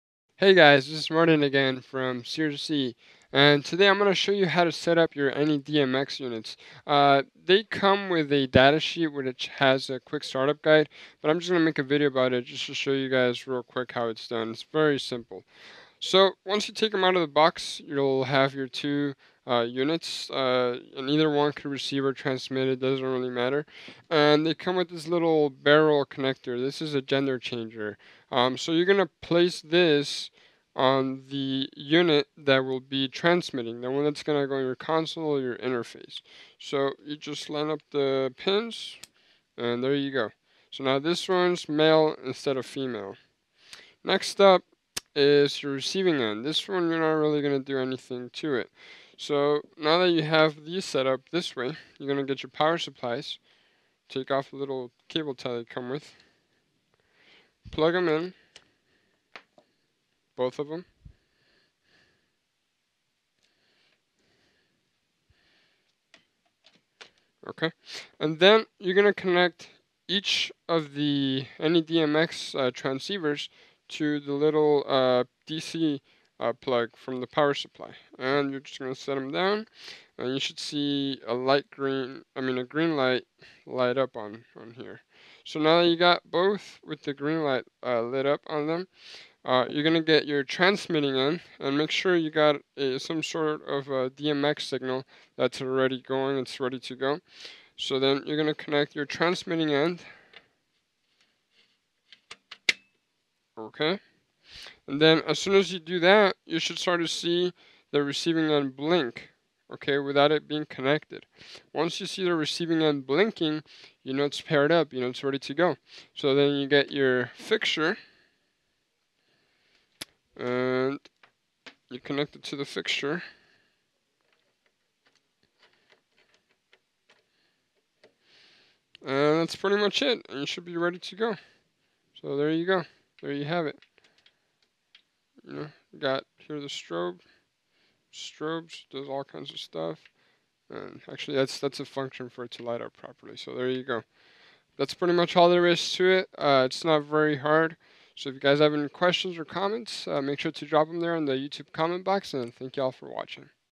anyDMX Setup Guide SIRS-E
In this video we will walk you through the pairing process of the AnyDMX. It is done in 7 easy steps:
1. Connect power supplies to each unit.
2. Select same color frequency and mode LED light using the frequency changing key on both units. (This is your operating frequency)
3. Connect transmitting unit to DMX console or software. (Gender changer is needed at this point)
4. The frequency and mode LED will begin to flash red on the unit connected as transmitter.
5. At this time, the frequency and mode LED on the receiving unit will flash green. (This means it's receiving)
6. Connect receiving unit to DMX fixture.
7. A successful connection has now been achieved.